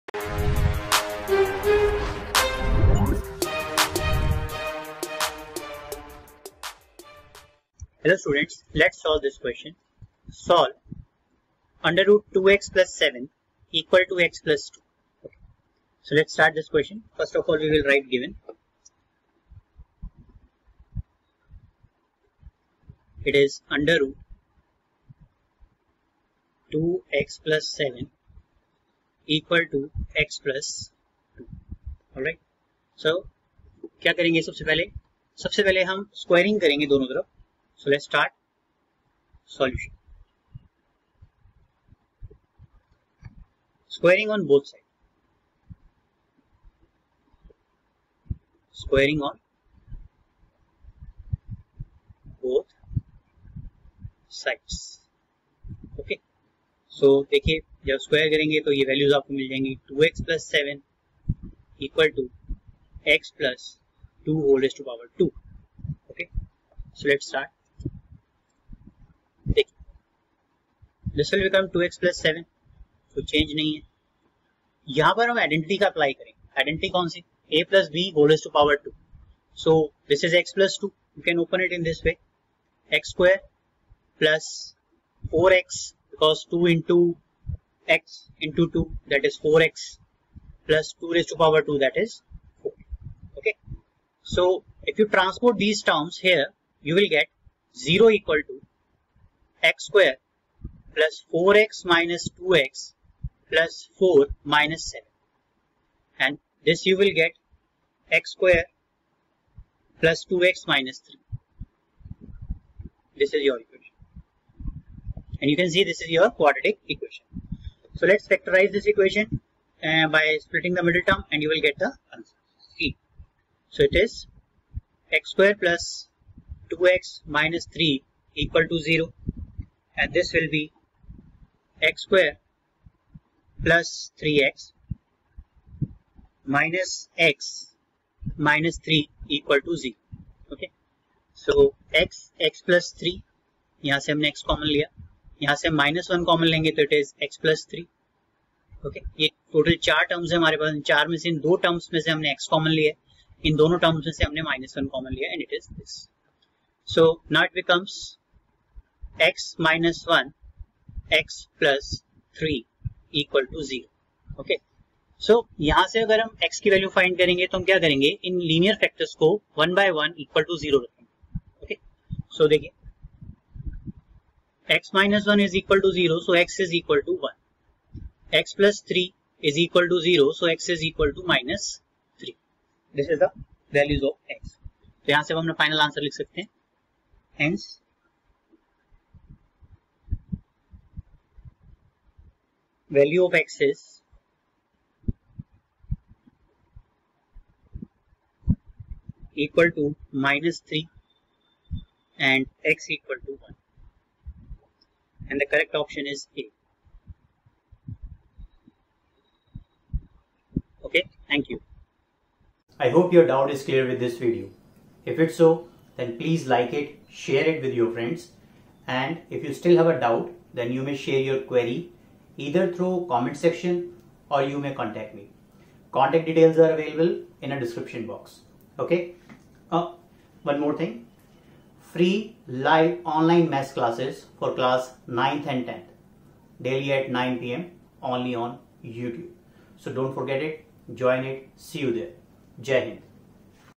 Solve √2x+7= x+2
Que. Solve √2x+7= x+2
From the Chapter: Ch 4 Class 10 Maths Quadratic Equations

1. Free Online Live Maths Classes Schedule
2. NCERT Textbook Video Solutions
3. Latest Updates Related To CBSE & Your Studies
4.Counselling Sessions
5.Free Study Material.